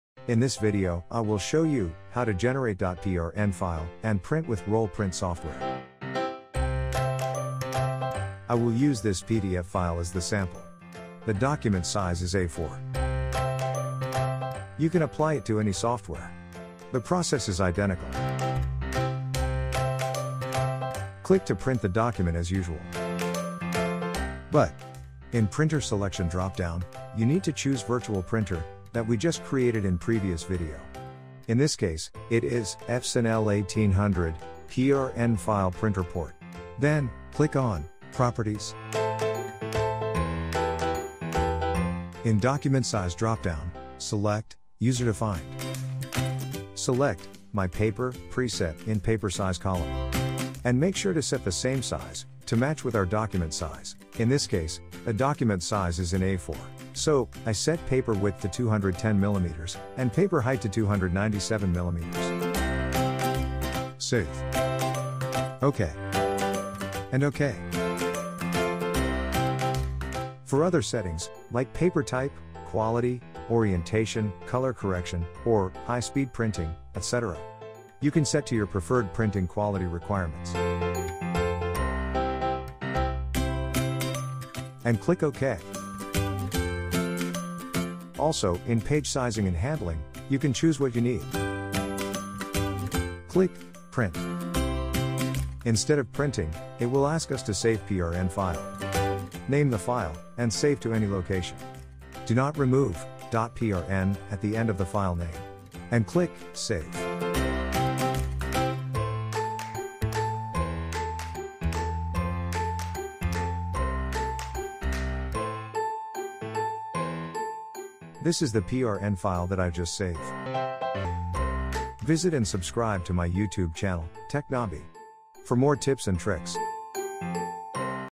How to Generate PRN File to Print with RollPrint Software
It's like you print as PDF, you choose to print as PRN with virtual printer. The process is identical to any software. PRN file is printing data to print with any software that support it such as RollPrint software to enable Epson desktop printers such as Epson L1300 or L1800 etc. to print in roll to roll printing.